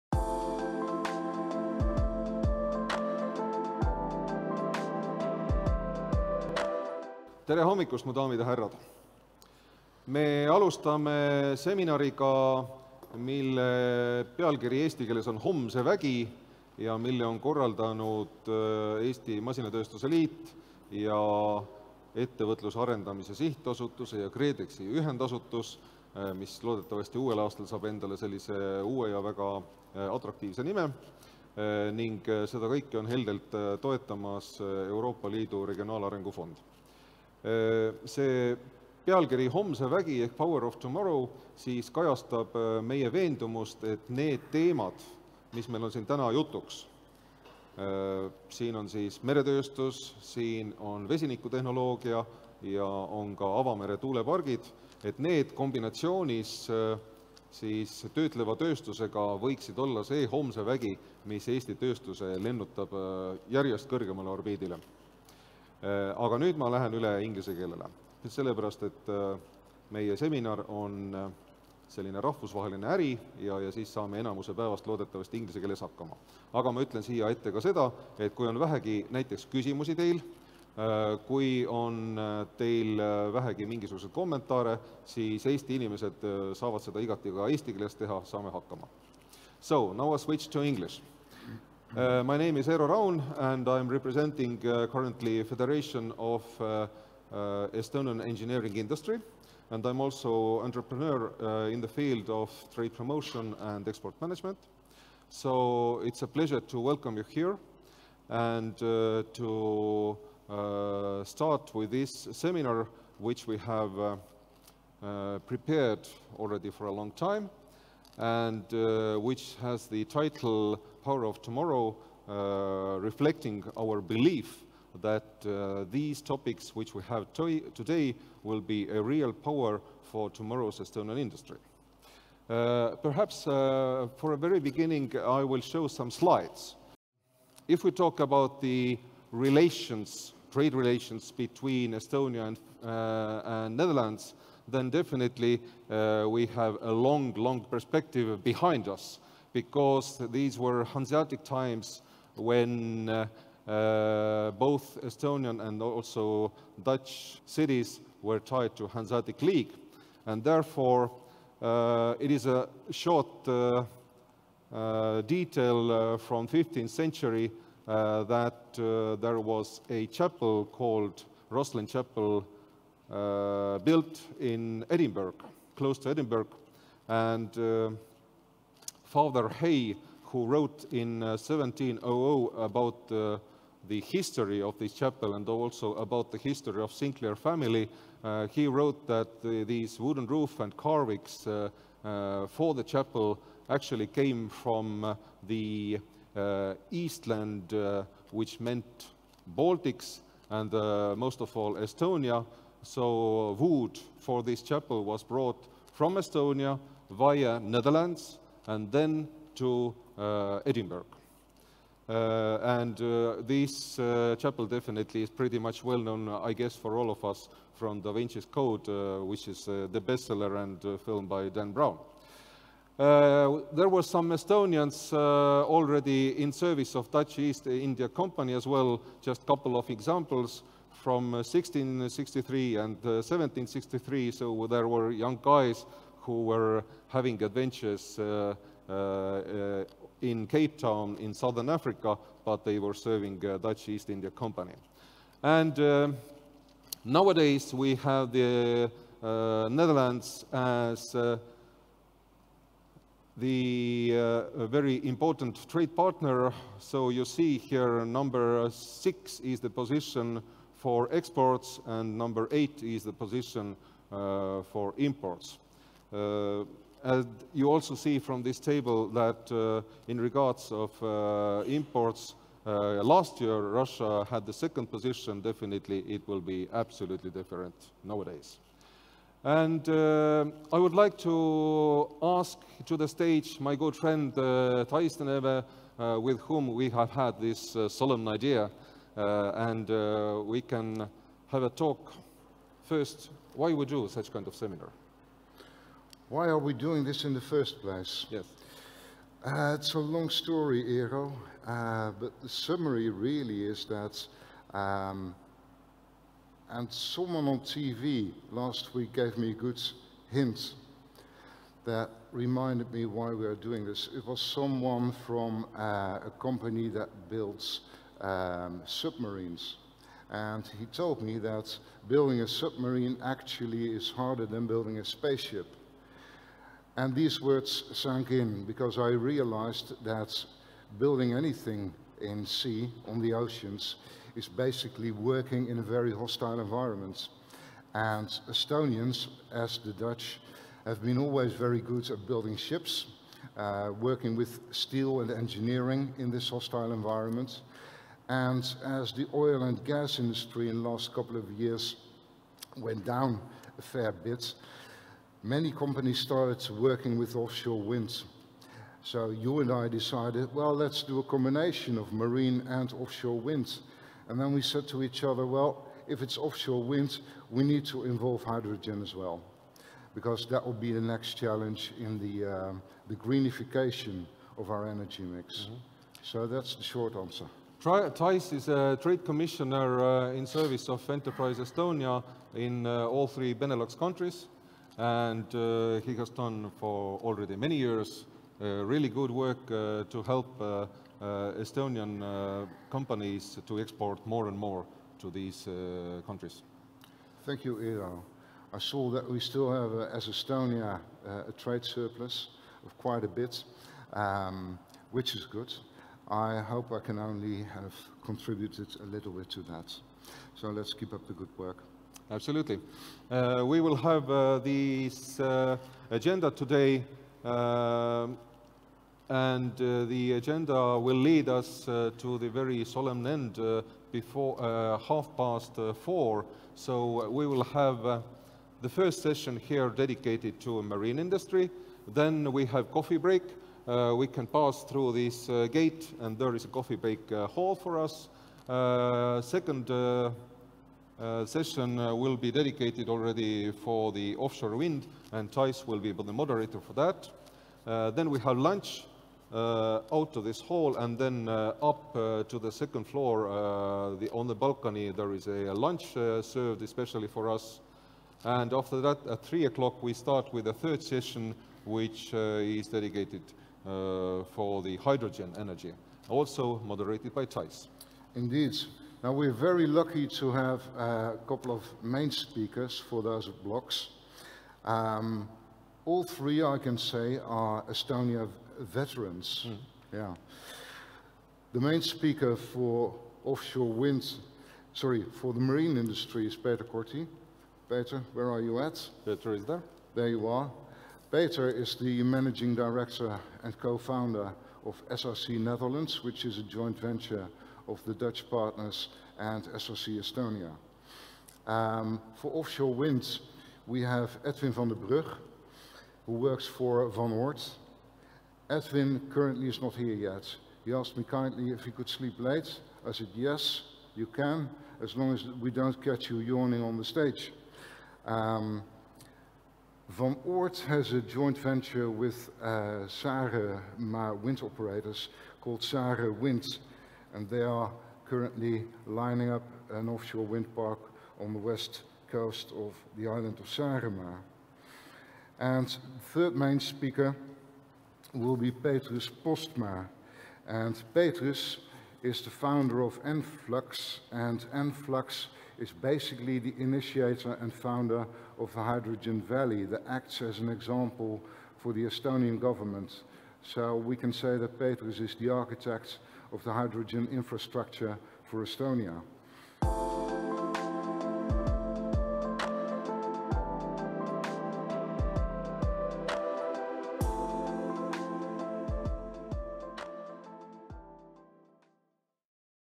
Opening Dutch-Estonian industrial seminar "Power of Tomorrow"
Thijs de Neeve
Trade Commissioner in Benelux, Enterprise Estonia (EAS)

Eero Raun
Development Adviser, Association of Estonian Engineering Industry